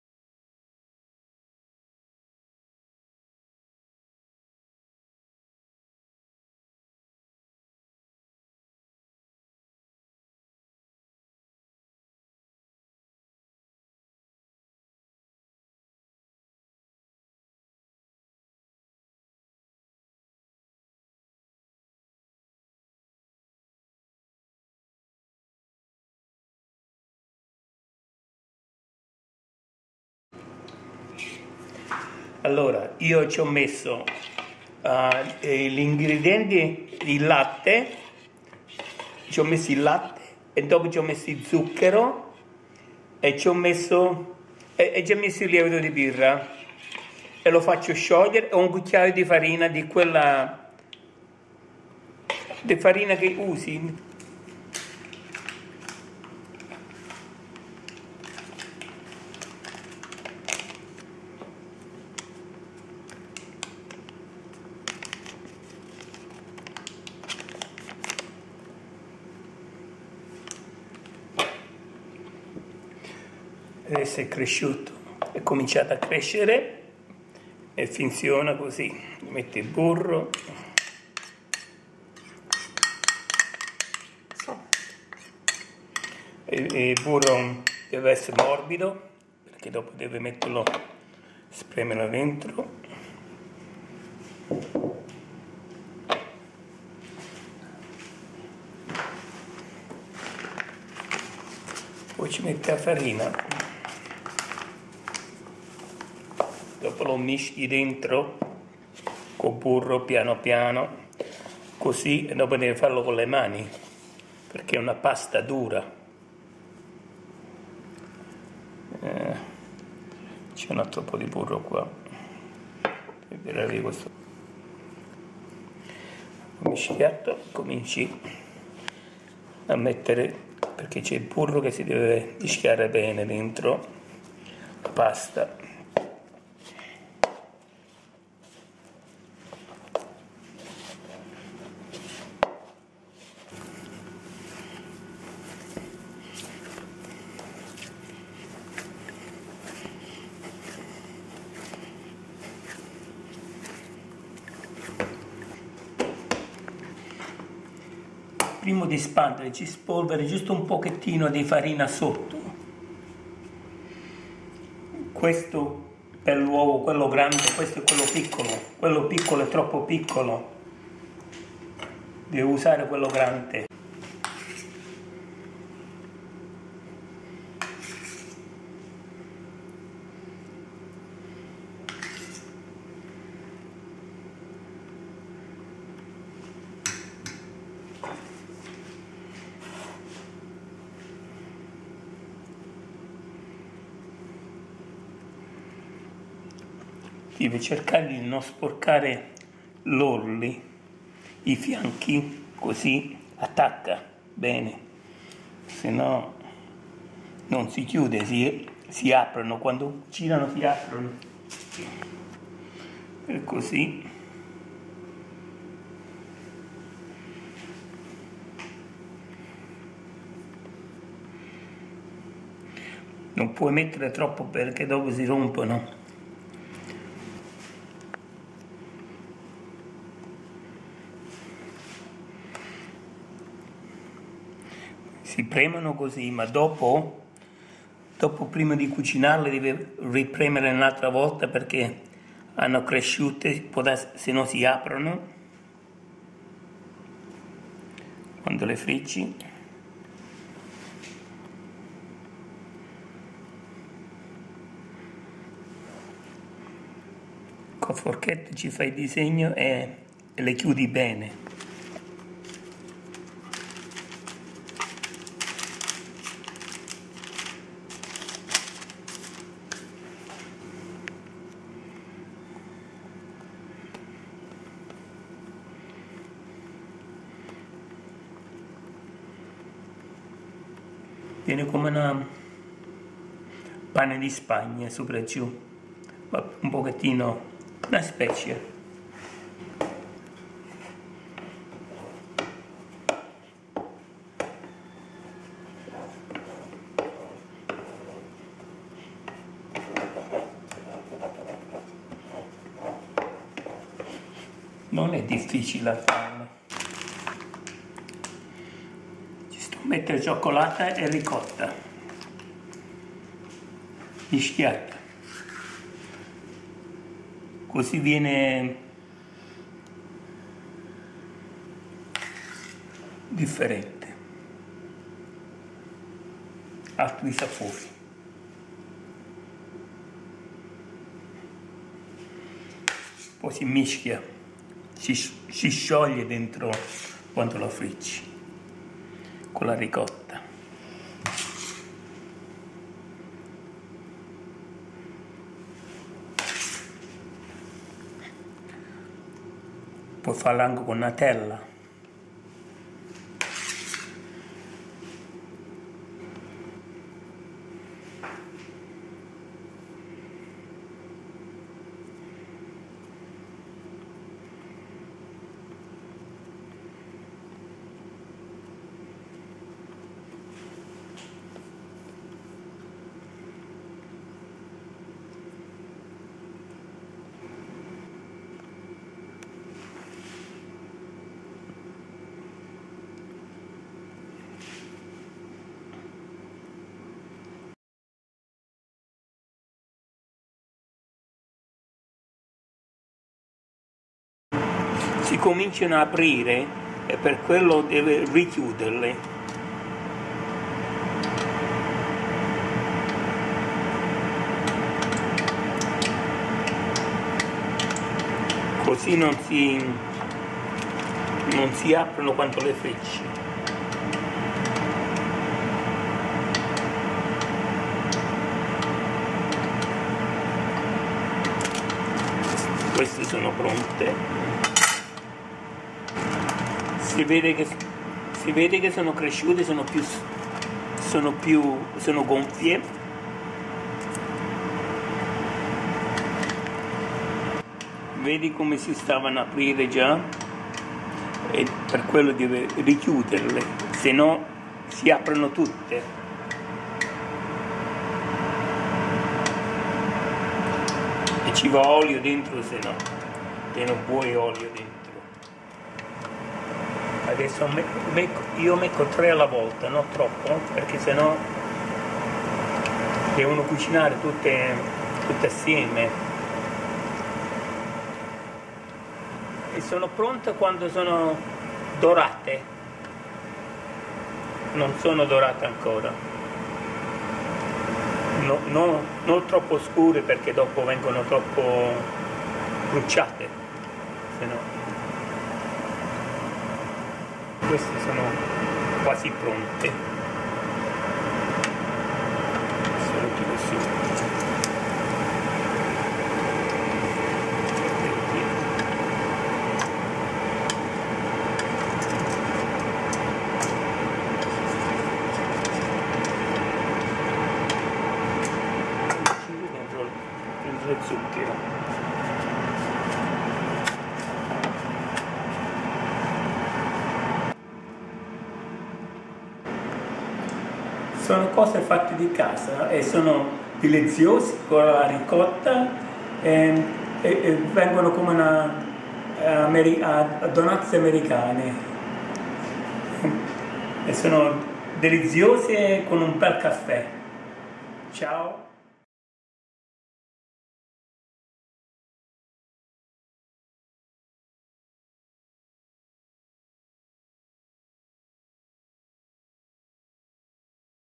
Dolce Dessert Mezzelune Ricotta Buone Soffice Nutella Pasqua Natale. Facile, tipo Cassateddi
Cucina di Fernando l'assecura la ricetta del dolce Mezzelune ripiene con la ricotta sono belle buone e soffice..una cosa facile a fare e deliziosa addata per ogni occasione usando meno lievto di birra. Mezzelune ripiene con la ricotta e nutella sono anche buone.
Try making these mouth watering sweets filled with ricotta or with ricotta and nutella they are just delicious using very little yeast and are easy to make. This sweet is so popular and loved by all. A sweet dessert for any occasion that tastes great even the next day...happy cooking from Cucina di Fernando